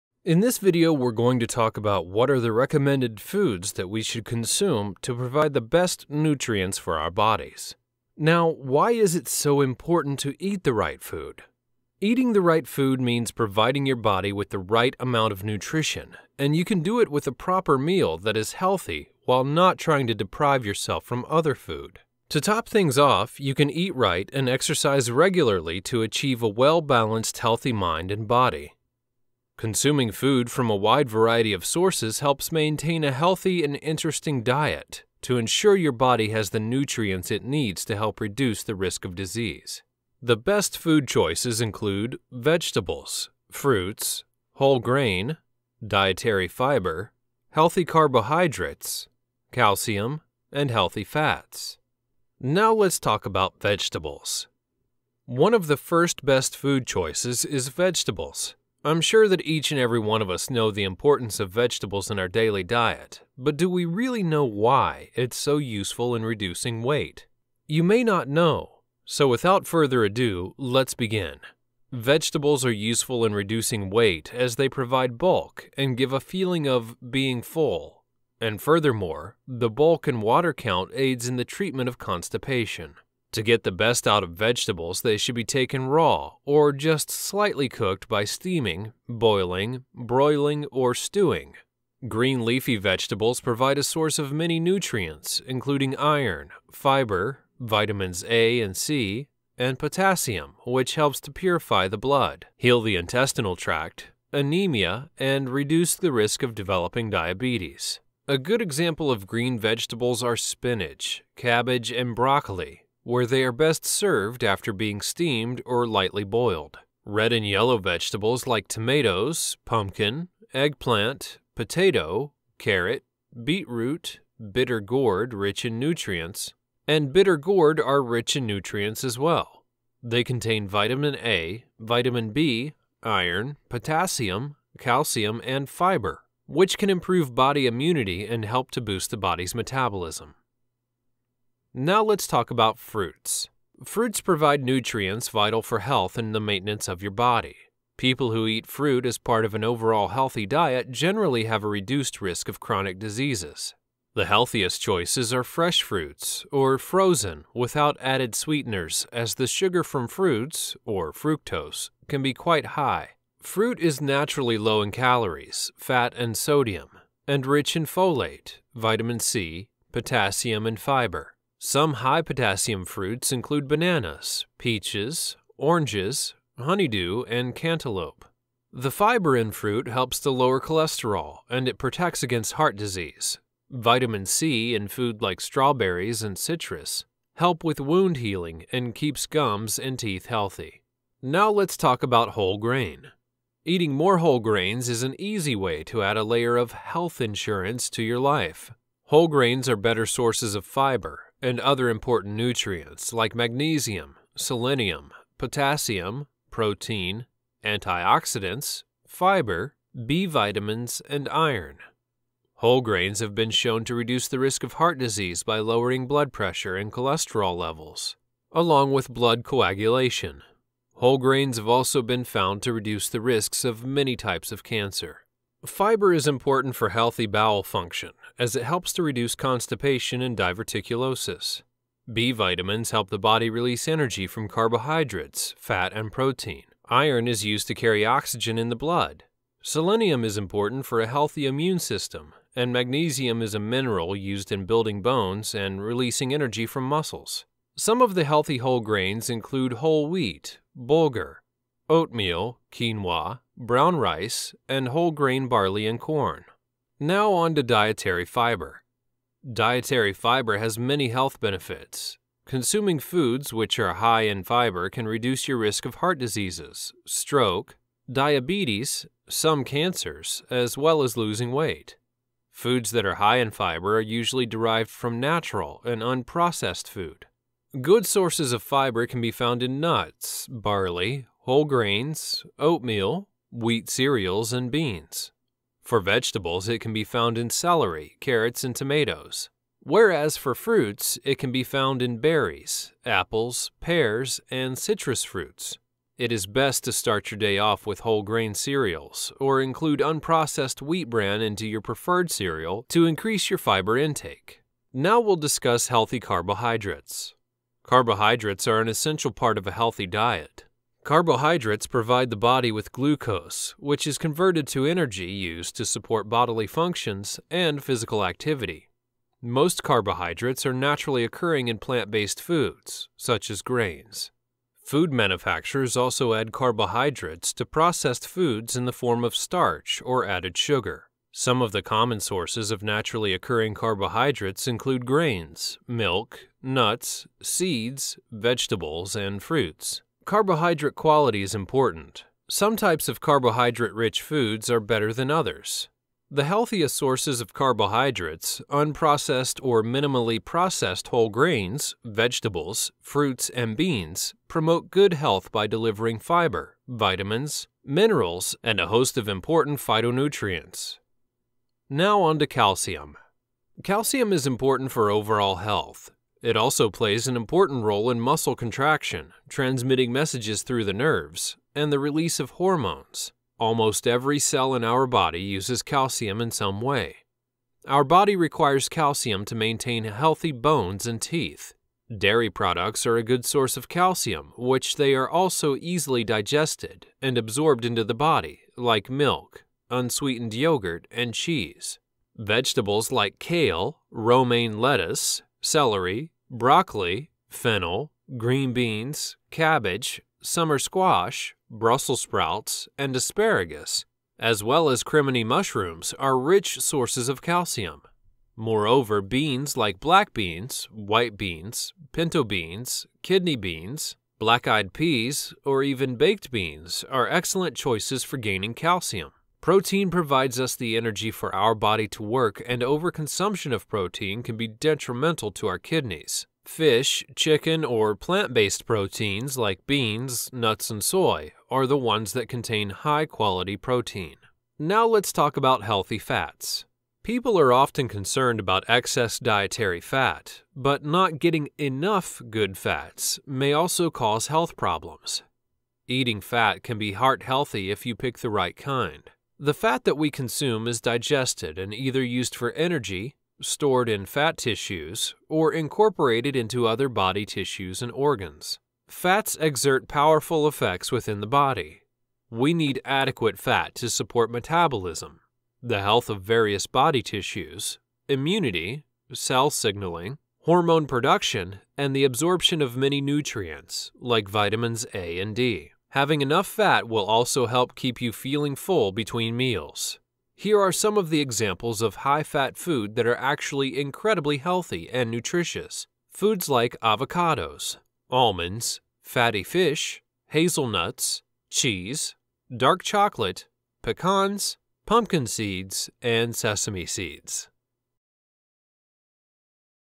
Healthy Eating | Types Of Recommended Food
Types of Recommended Food
Good nutrition is an important part of leading a healthy lifestyle. To top things off you can eat right and exercise regularly to achieve a well-balanced healthy mind and body. Consuming food from a wide variety of sources helps maintain a healthy and interesting diet and ensure your body has the nutrients it needs to help reduce the risk of disease.